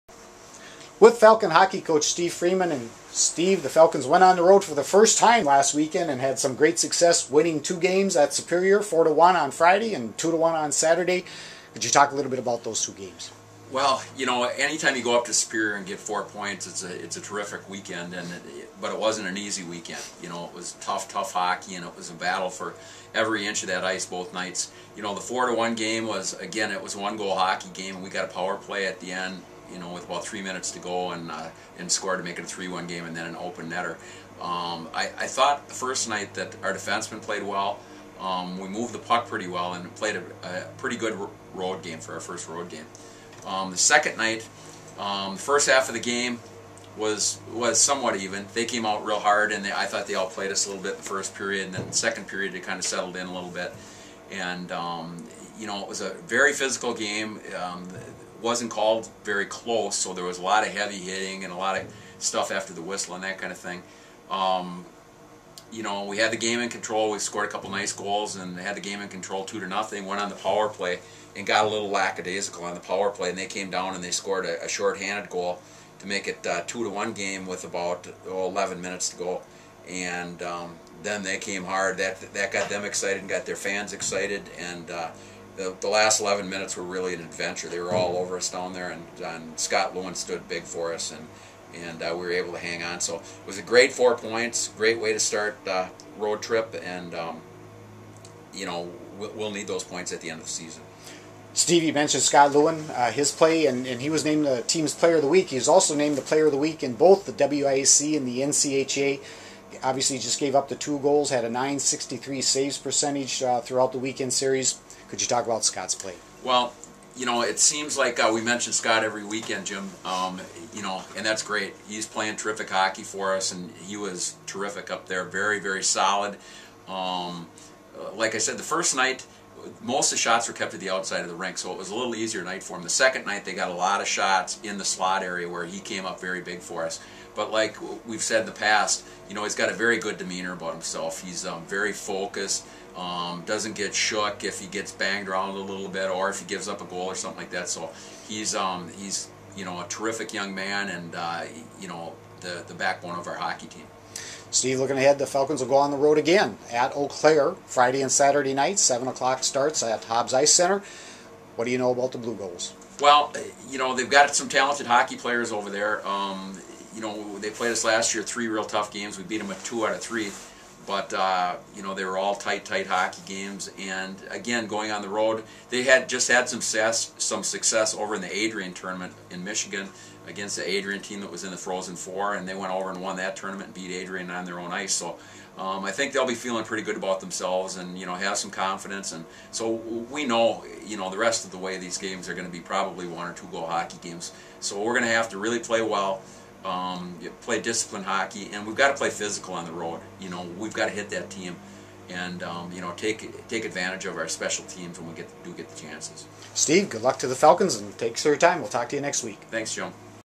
UW-River Falls Men's Hockey - Steve Freeman Interview - Dec. 8, 2011
Falcon Coach Steve Freeman talks about last weekend's series at UW-Superior, Player of the Week Scott Lewan and this weekend's series at UW-Eau Claire